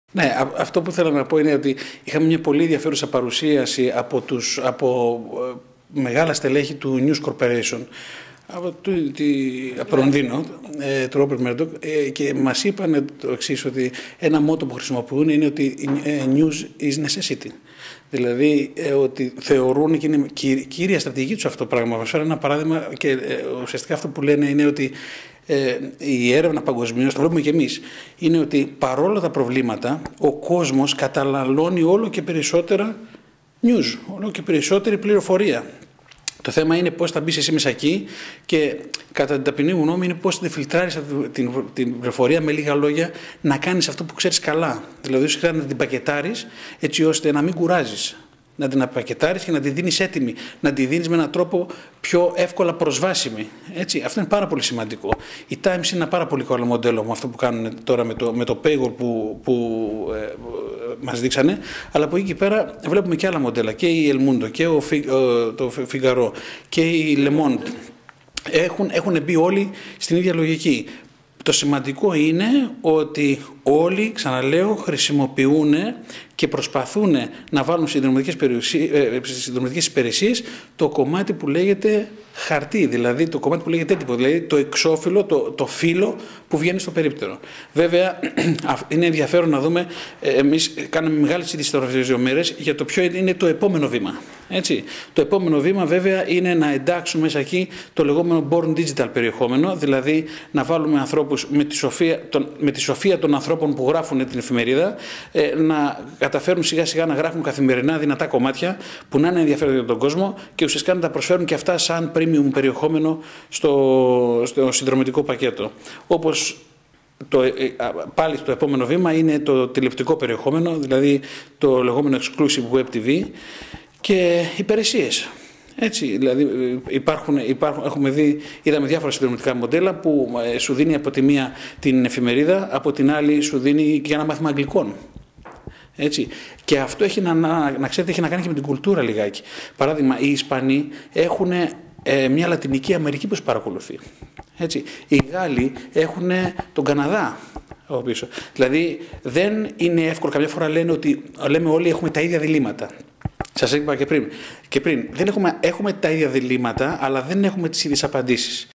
Nikos Gouraros - Feed people with 'delicious' info [English Subtitles]
This video interview has been recorded by QJNT 4 at the European Online Publishers Association Conference hosted by the "Quality Journalism & New Technologies" M.A. program in Donau, Krems, Αustria in February-March 2013.
QJNT is a joint graduate program of European public higher education and research entities